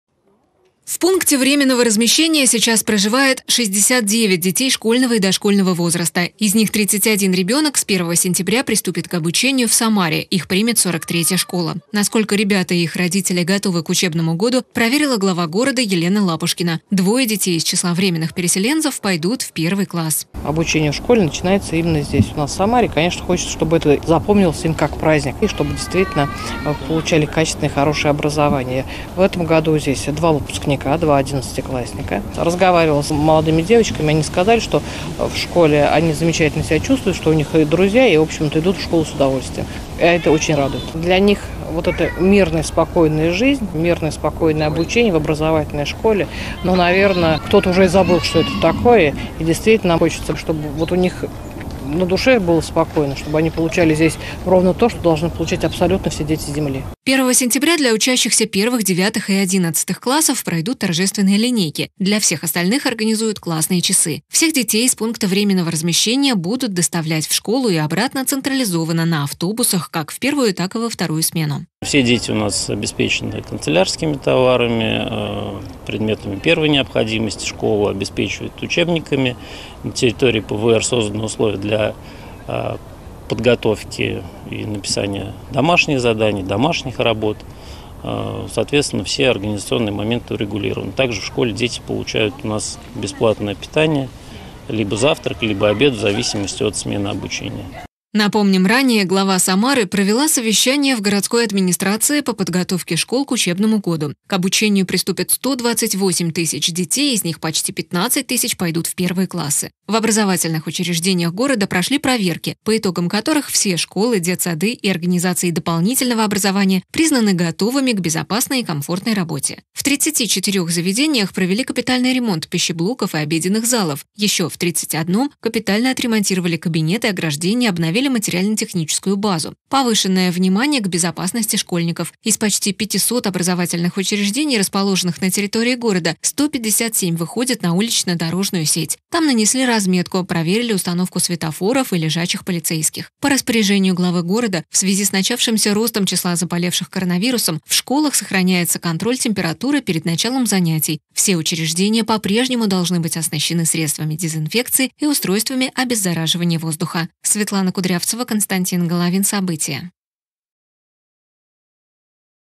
Елена Лапушкина проверила, готовы ли дети из пункта временного размещения к новому учебному году
В Самаре 128 тысяч детей пойдут в школу. Среди них - и ребята с территории Донбасса, которые сейчас находятся в пункте временного проживания. Некоторые из них приехали еще зимой, для них образовательный процесс продолжится, а часть детей в самарскую школу пойдут впервые, двое -  в первые классы. Готовы ли вынужденные переселенцы к началу учебного года, проверила глава Самары Елена Лапушкина, подробности - в сюжете.